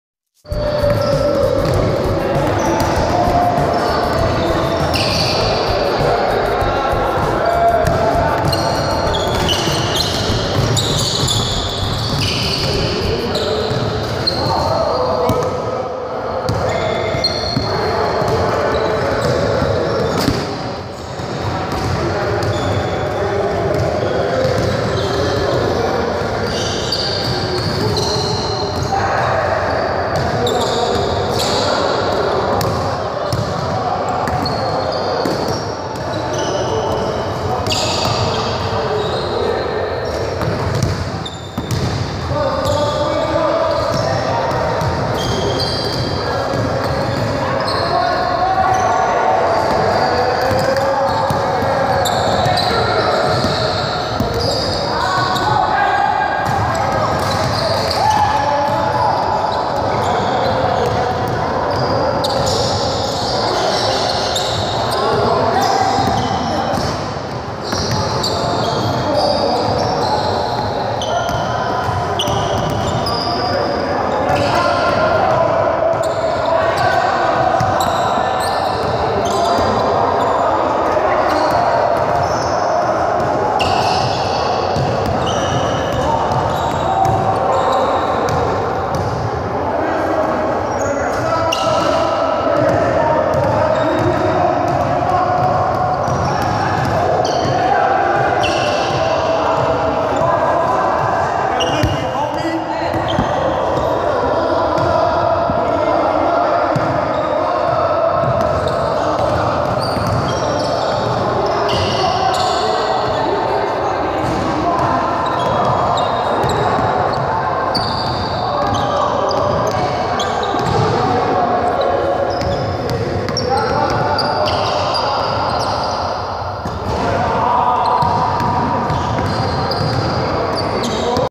Paranormal clip of a ghost try not. Too get scared
This video  contains  a man just  playing basketball  but look  too your right it looks  like a man slowly  transforming into a demon this is the footage